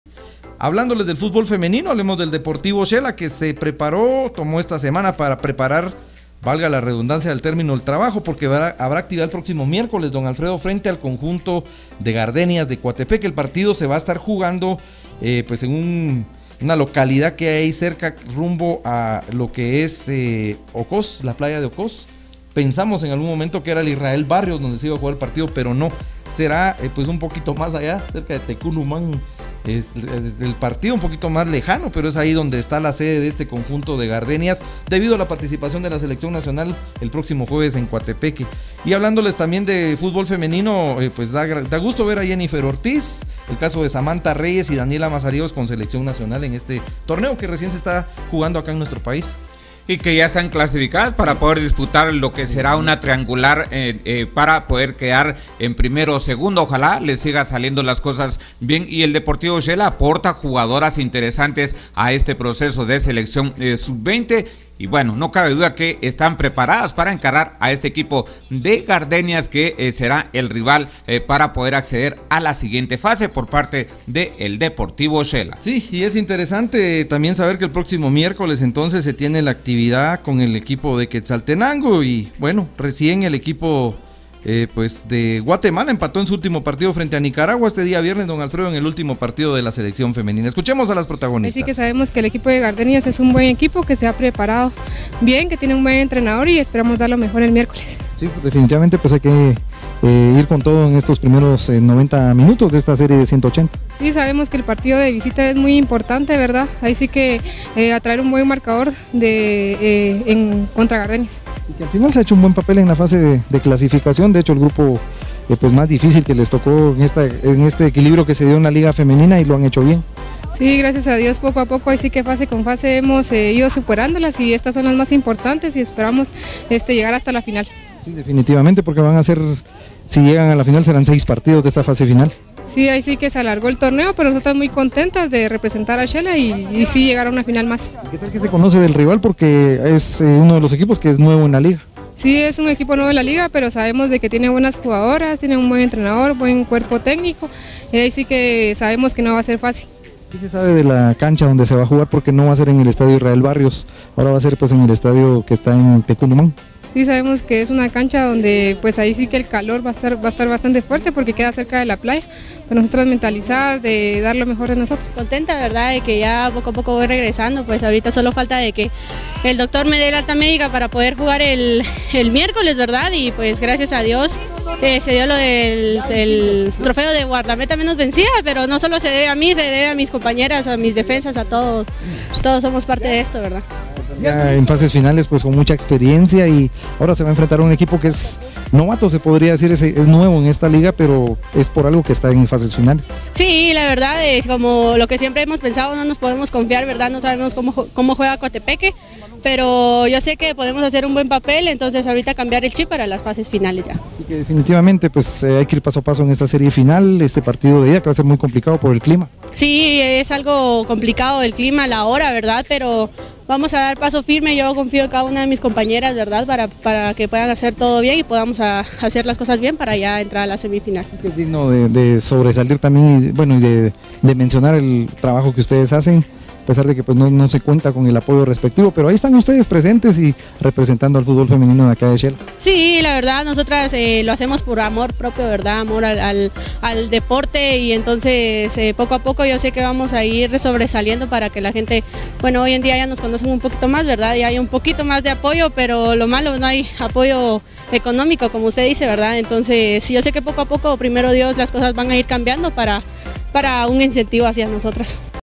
Deportivo Xela visitará a Gardenias Cuartos de Final AP2019
Deportivo Xela se prepara para visitar a Gardenias
Cuartos de Final  juego de ida
Miércoles 20 noviembre   12:00 horas
Estadio Florencio Barrios, La Blanca San Marcos